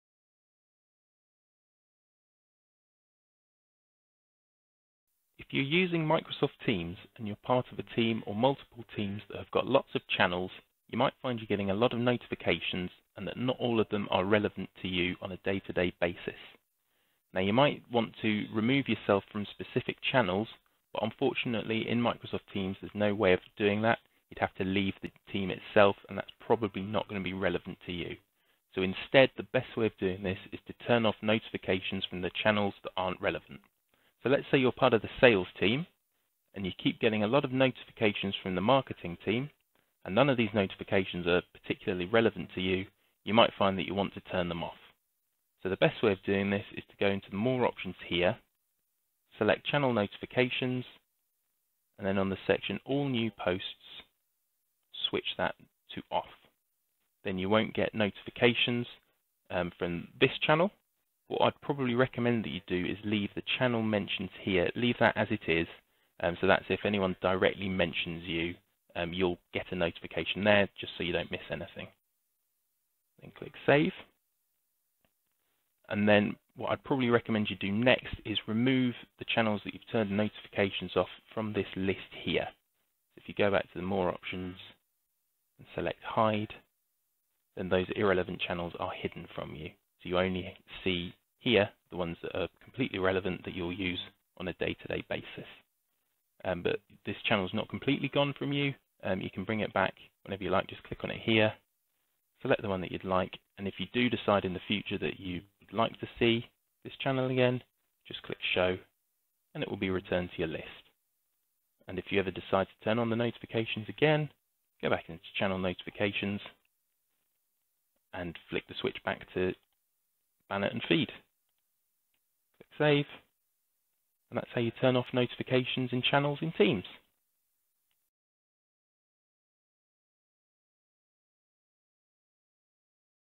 How to turn off channel notifications in Microsoft Teams
Find out how to turn off channel notifications in Microsoft Teams

ACUTEC Limited
St Peters House, Church Hill, Coleshill, B46 3AL